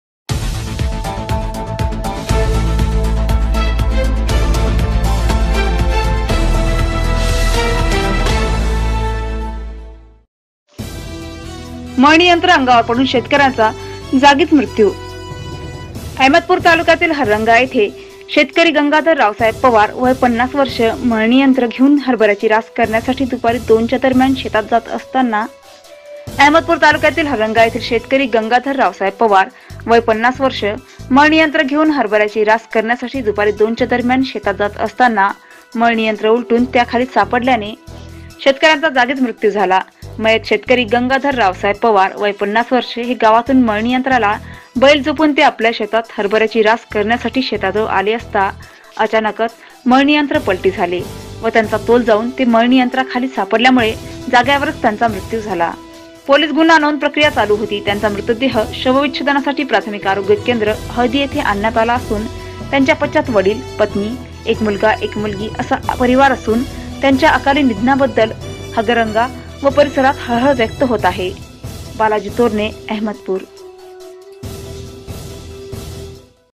मळणी यंत्र अंगावर पडून शेतकऱ्याचा जागीच मृत्यू !
अहमदपूर तालुक्यातील हंगरगा येथील शेतकरी गंगाधर रावसाहेब पवार वय 50 वर्ष मळणी यंत्र घेऊन हरभऱ्याची रास करण्यासाठी दुपारी दोनच्या दरम्यान शेतात जात असताना मळणी यंत्र उलटून त्याखाली सापडल्याने शेतकऱ्यांचा जागीच मृत्यू झाला मयत शेतकरी गंगाधर रावसाहेब पवार वय पन्नास वर्ष हे गावातून मळणी यंत्राला बैल जुपुन ते आपल्या शेतात हरभऱ्याची रास करण्यासाठी शेताजवळ आले असता अचानकच मळणी यंत्र पलटी झाले व त्याचा तोल जाऊन ते मळणी यंत्रा खाली सापडल्यामुळे जाग्यावरच त्यांचा मृत्यू झाला पोलीस गुन्हा नोंद प्रक्रिया चालू होती त्याचा मृत्यू देह शवच्छेदनासाठी प्रथमिक आरोग्य केंद्र हळदी येथे आणण्यात आला असून त्यांच्या पश्चात वडील पत्नी एक मुलगा एक मुलगी असा परिवार असून त्यांच्या या अकाली निधनाबद्दल हंगरगा व व परिसरात हळहळ व्यक्त होत आहे...बालाजी तोरणे.अहमदपूर